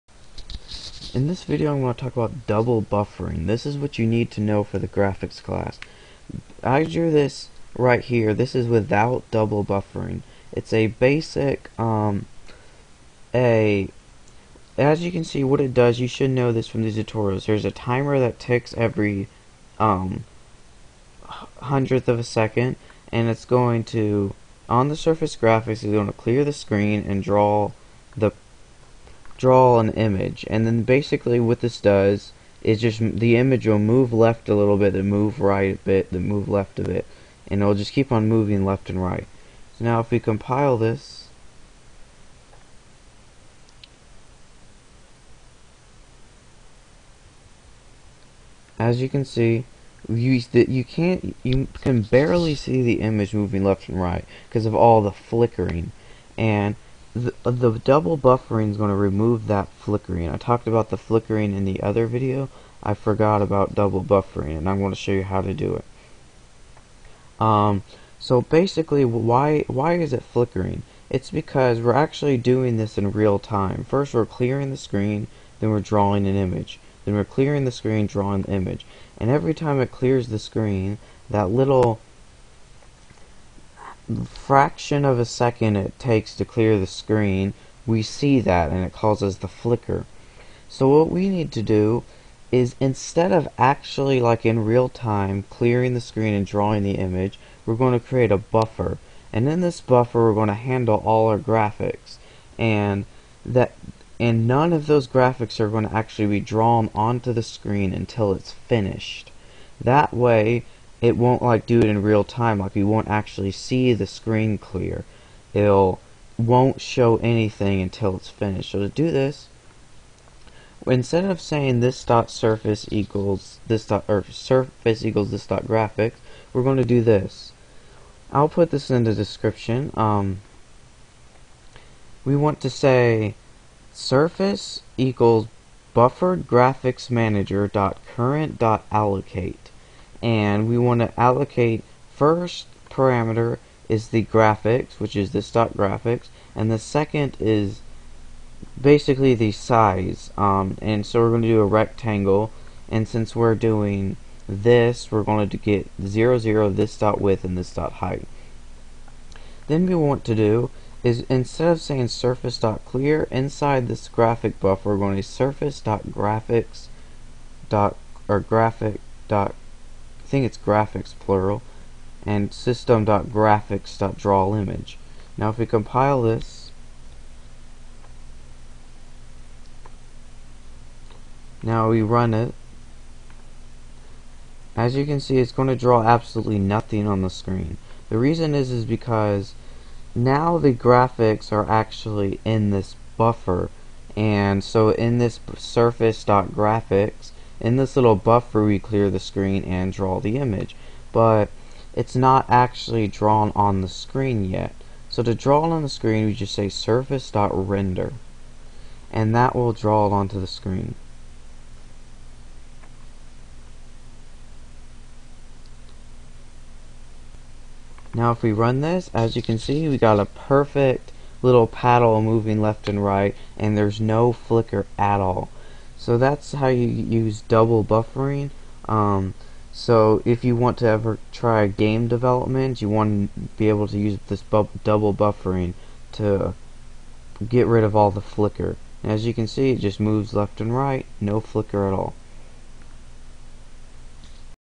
JScript .NET Tutorial 11 - Double Buffering For Graphics
With this you can really create any 2D game you want.

var surface = BufferedGraphicsManager.Current.Allocate(this.CreateGraphics(), new Rectangle(0,0,this.Width,this.Height));